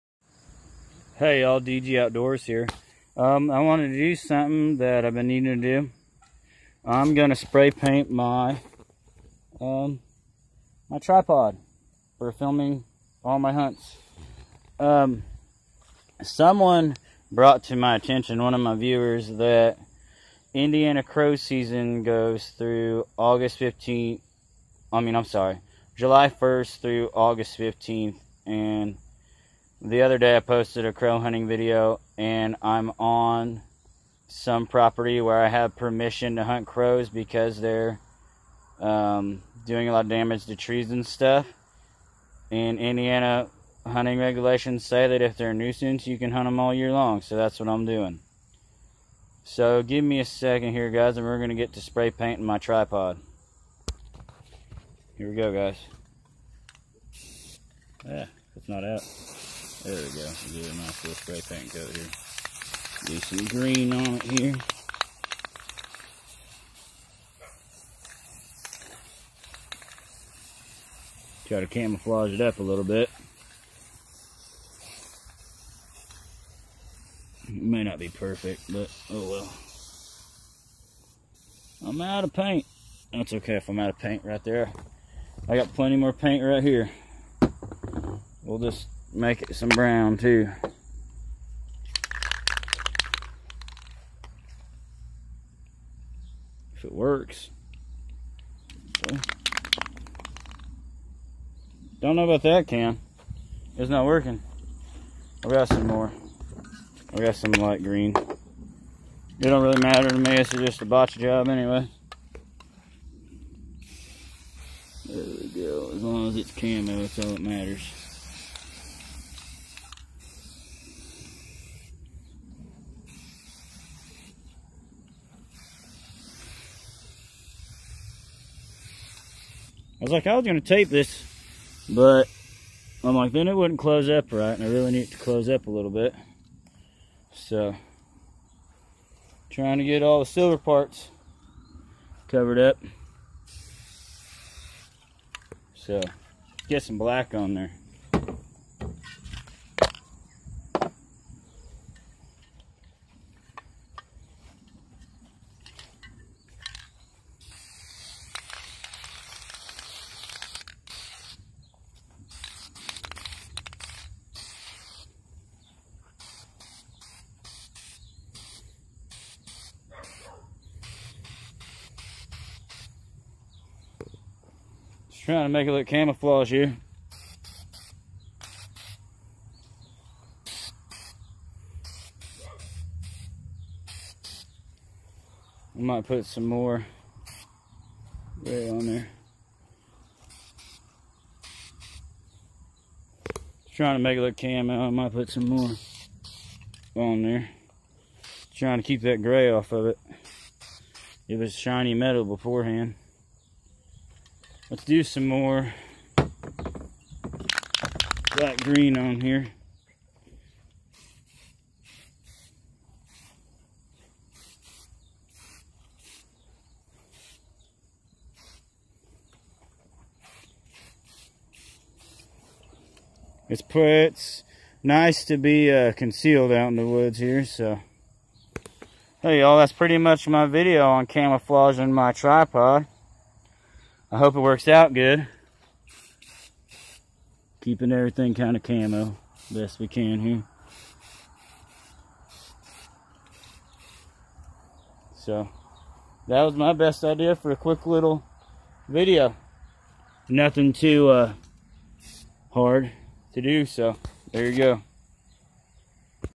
INDIANA CROW HUNTING REGULATIONS/ SPRAY PAINTING MY CAMERA TRIPOD CAMO!!!!!!
Thanks to my viewer( a good friend of mine ) I clarify the crow hunting regulations in Indiana and spray paint my filming tripod camo